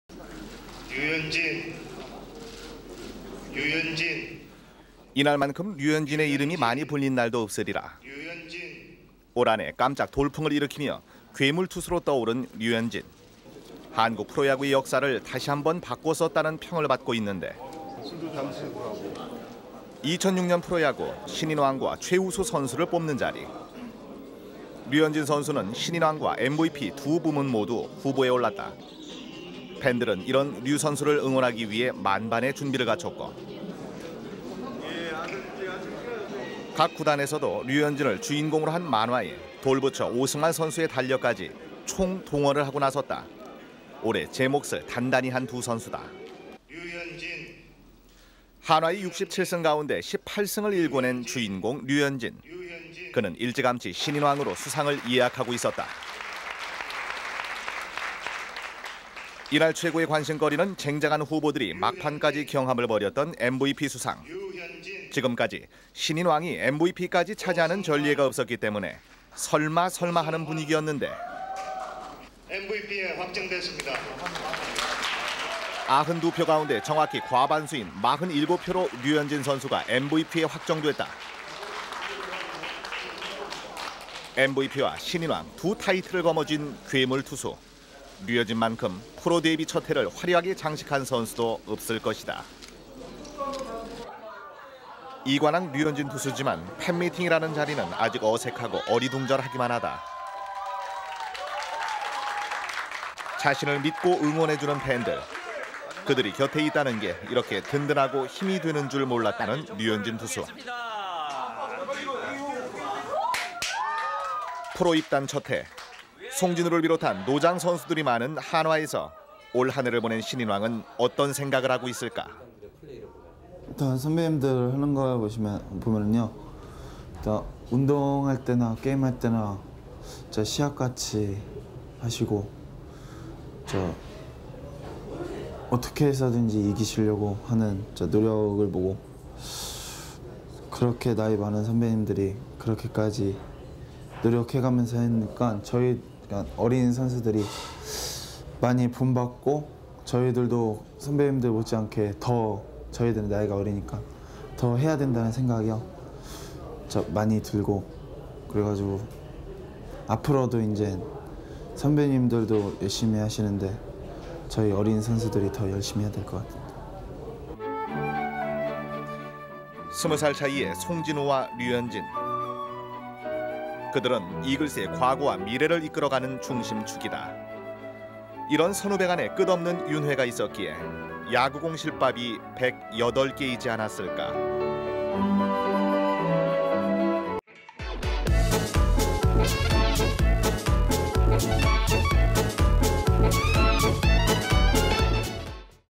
【TJB 아카이브】 그때 그 시절 겁 없던 괴물 신인 류현진⚾🤴 아기 시절 류현진의 과거 영상을 털어보자😋
【TJB 아카이브】
그때 그 시절 겁 없던 괴물 신인 류현진⚾🤴
아기 시절 류현진의 과거 영상을 털어보자😋